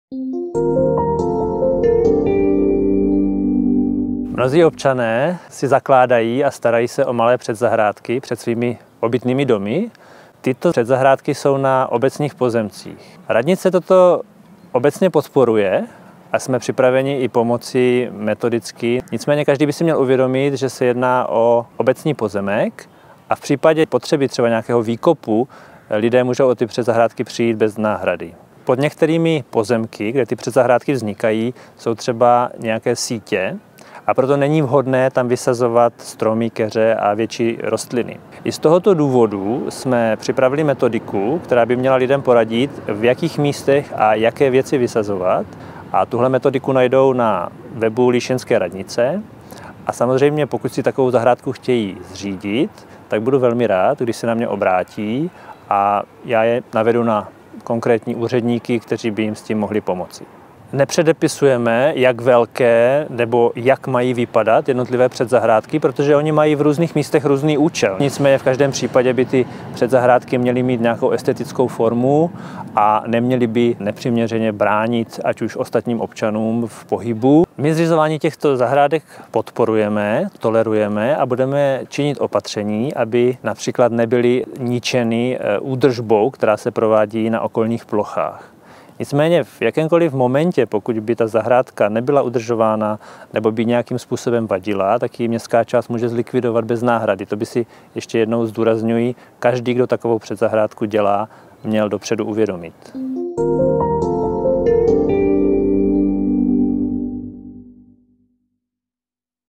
Radnice podporuje vznik předzahrádek
Na obecních pozemcích před bytovými domy si řada lidí z Líšně zřizuje malé zahrádky. Radnice jejich snahu podporuje a připravila společně s odborníky metodiku, jak by tyto předzahrádky měly vypadat a pomáhá s jejich vznikem. Přesto je nutné si uvědomit, že pod zemí mnohdy vedou inženýrské sítě a jejich správci k nim potřebují mít zajištěný odpovídající přístup.